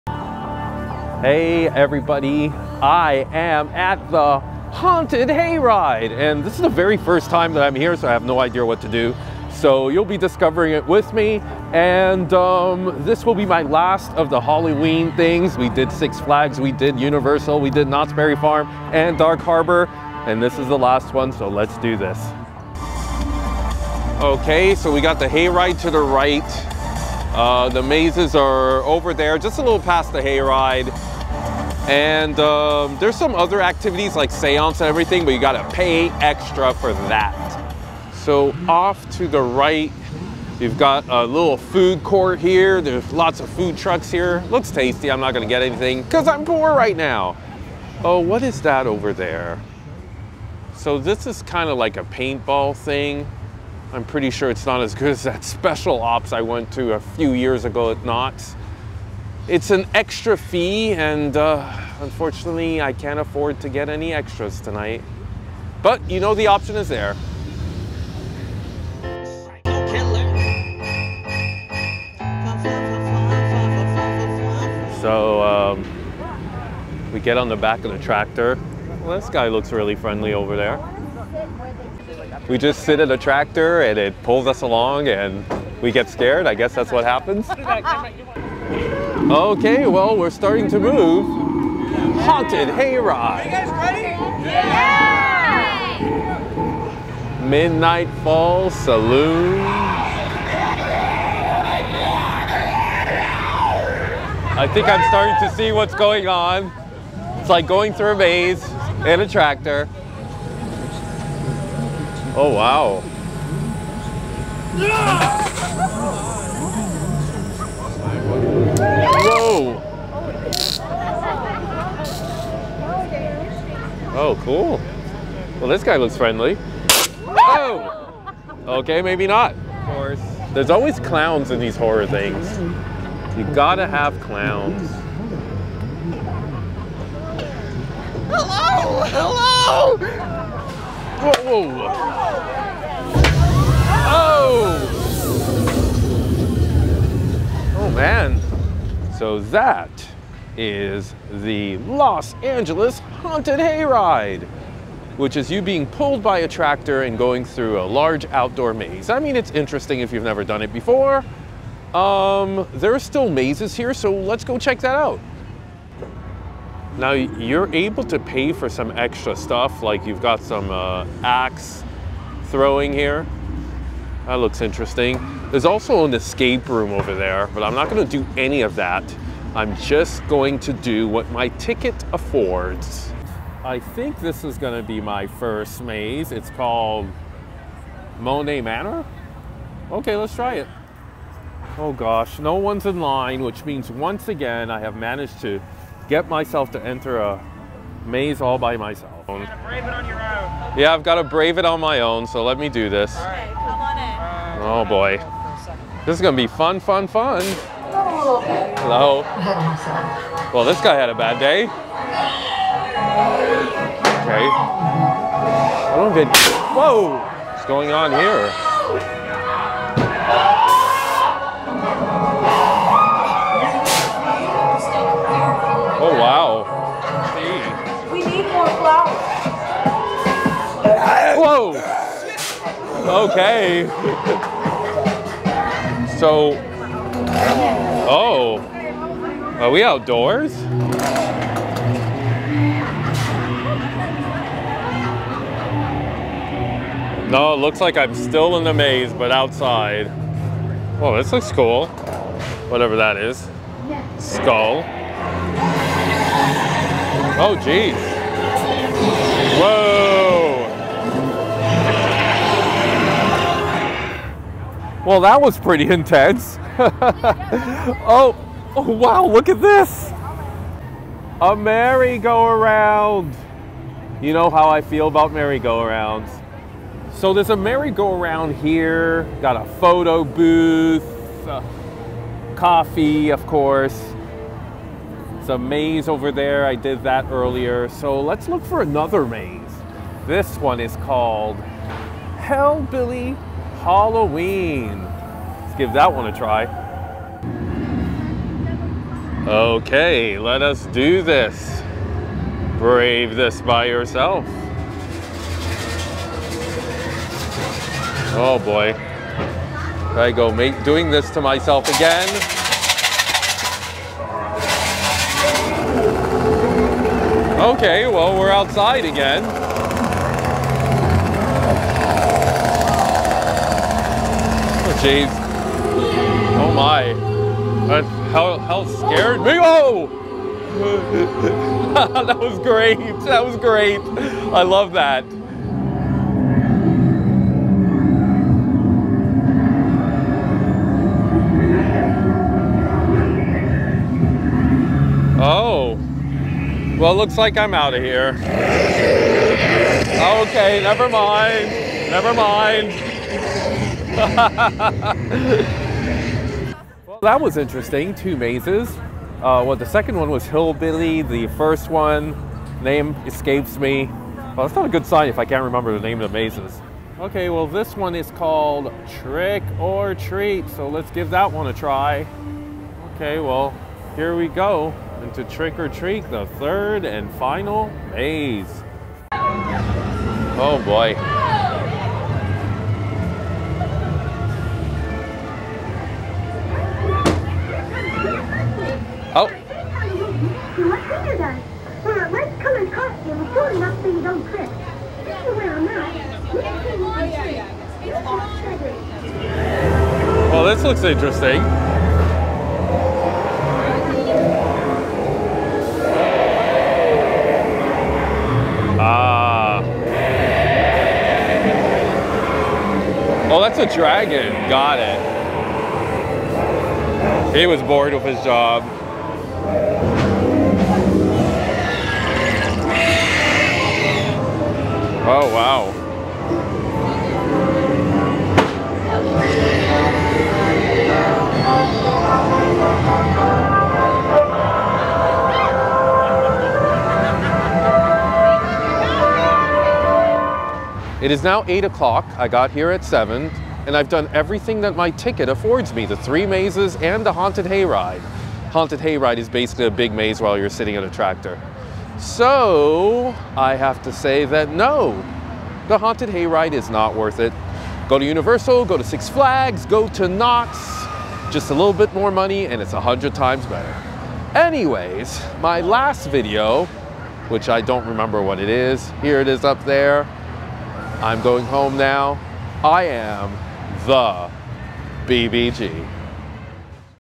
LA Haunted Hayride 2024 + All Mazes
I went to the LA Haunted Hayride for the very first time. What did I think?
INDEX
1:15 Haunted Hayride
3:03 1st Maze Manoe Manor
4:50 2nd Maze Hellbilly Halloween
6:06 3rd Maze Trick or Treat

Yes, there are only 3 mazes.